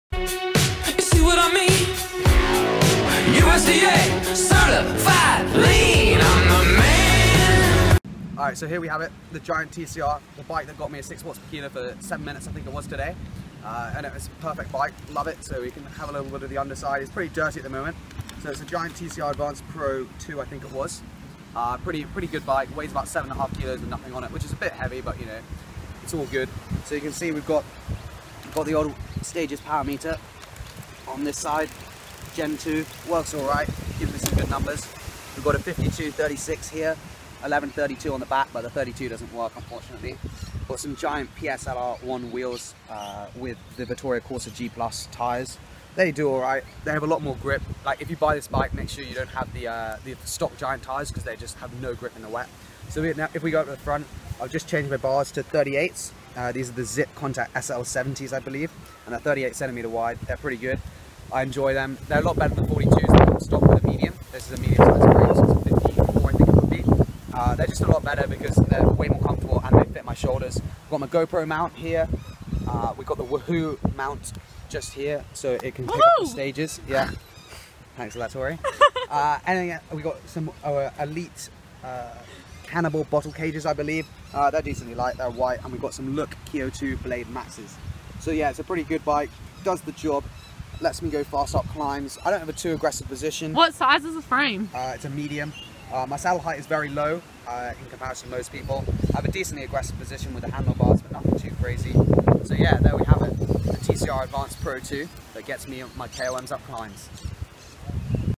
My KOM Hunting Bike
The bike I ride to get my KOM's. After doing 6w/kg for 8 mins up Mt Osmond I decided to show you guys what bike I ride. Strava